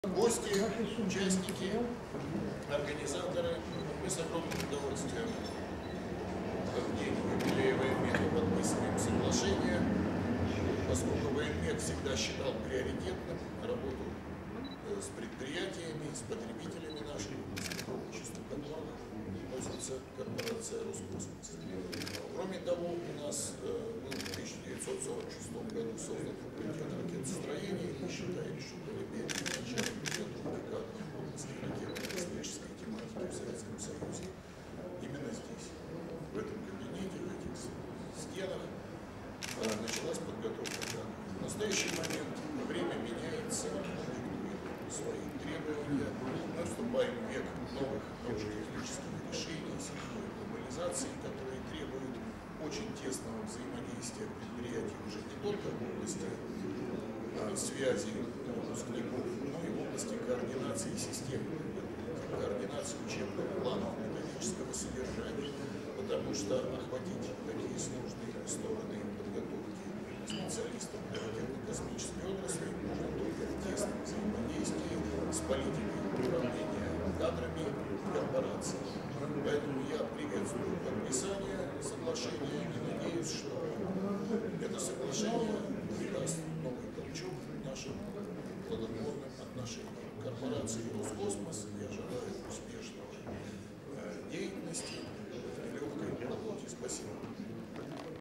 подписание Соглашения между БГТУ «ВОЕНМЕХ» им. Д.Ф.Устинова и Государственной корпорацией по космической деятельности России (Роскосмос). По окончанию церемонии подписания состоится пресс-брифинг для журналистов, в котором прим